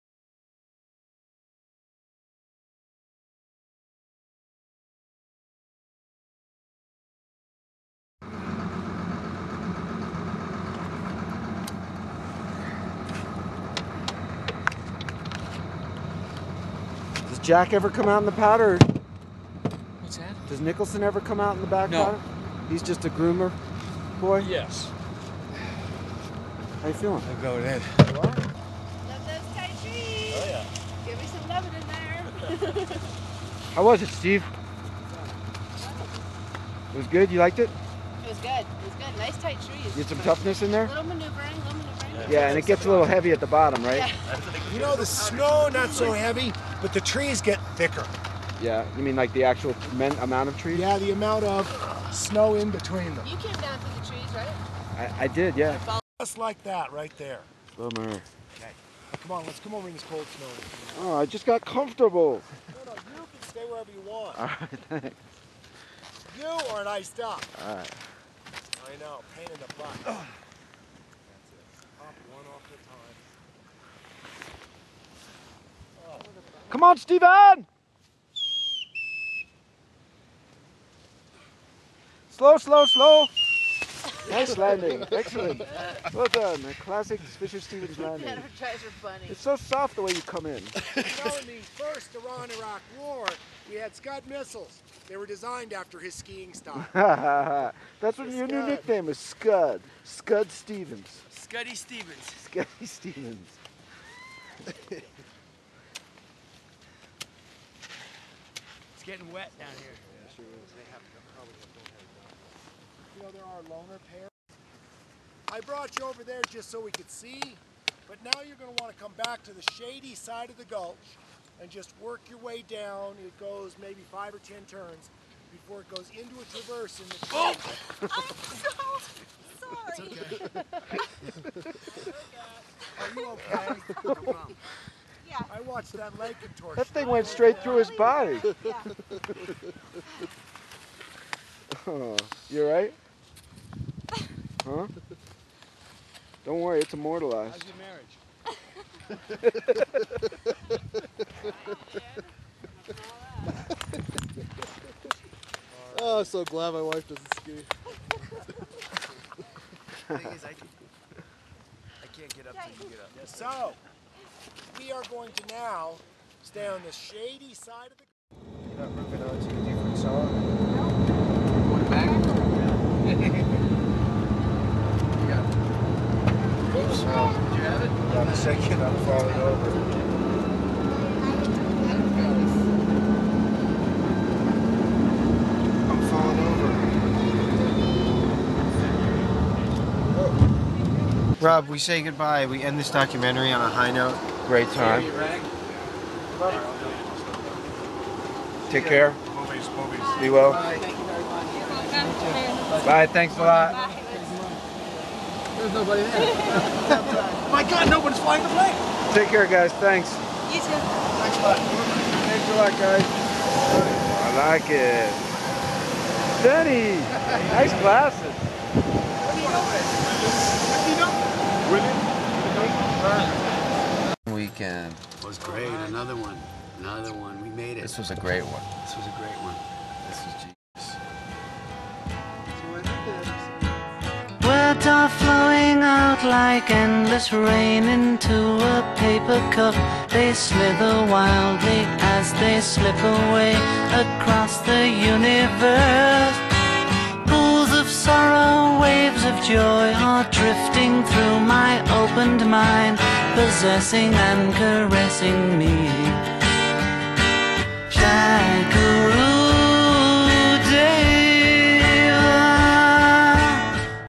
"The Forgetful Fish" (part 7) - Rob Morrow, Fisher Stevens in Aspen
Rob Morrow and Fisher Stevens at the Aspen Youth Experience Celeb ski event 2009